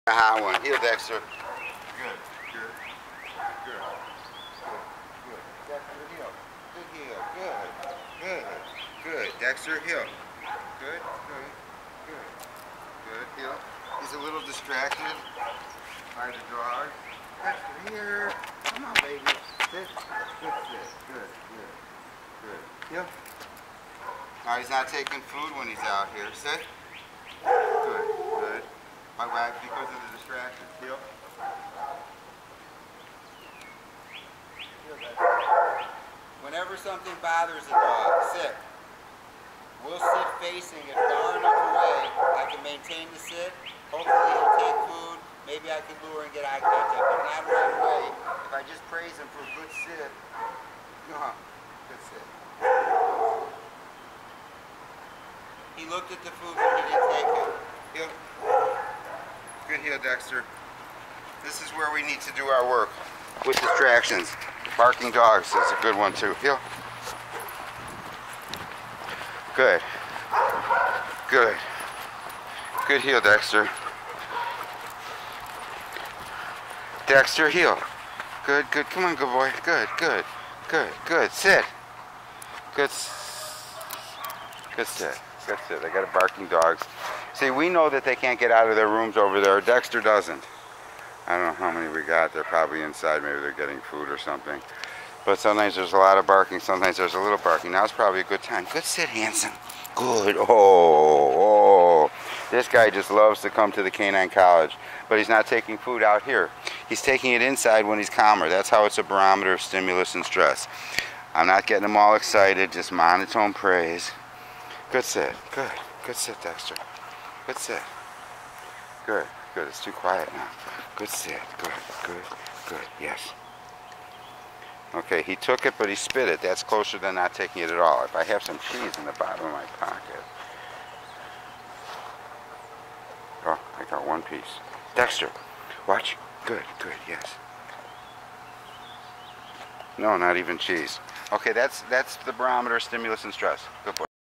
Allan Ross Dog Training
Dexter M. is a 2.6 year old Chocolate Lab in Board and Train.  Here we practice obedience in front of the Canine College.  He is bothered by the barking dogs and stops taking food.  Food is a barometer of stimulus and stress.  This means we need to socialize and desensitize with anything he is reactive to.